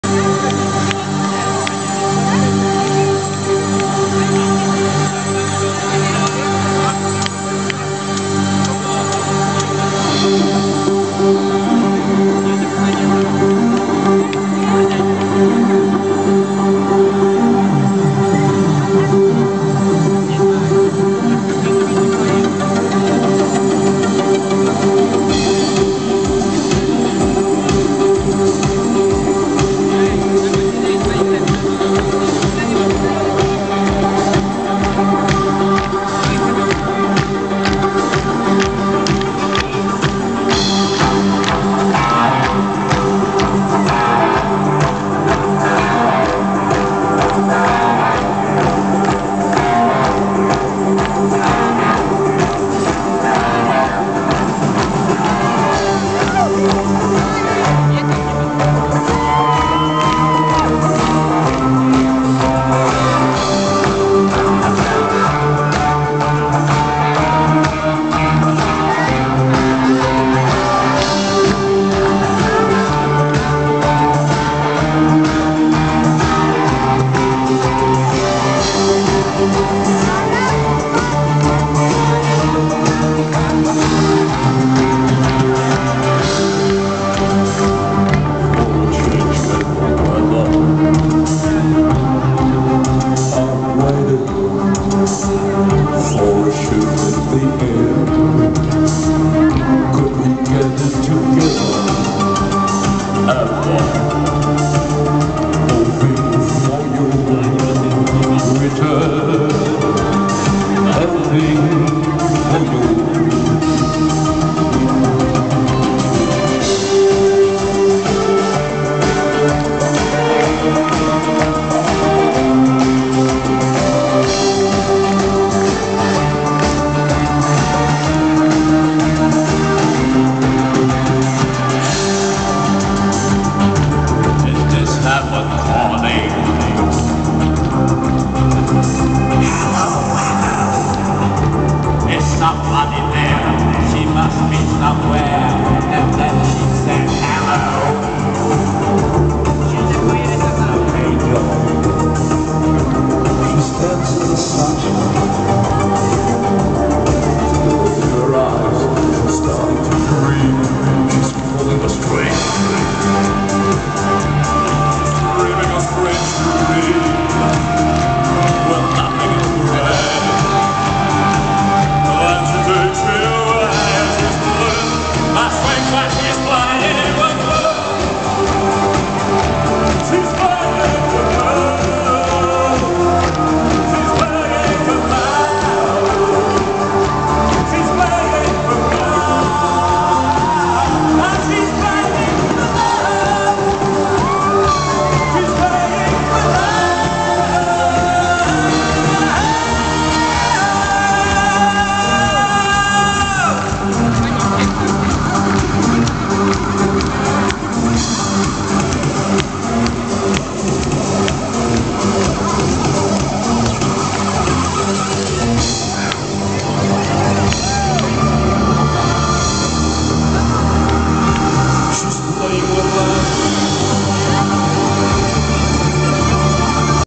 Alphaville (4) koncert (Strefa Kibica Poznań - Euro 2012) / Concert - Poznan Fan Zone
Koncert zespołu Alphaville w Strefie Kibica w Poznaniu (Euro 2012).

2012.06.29

Alphaville group concert in Poznan Fan Zone (Poland, Euro 2012).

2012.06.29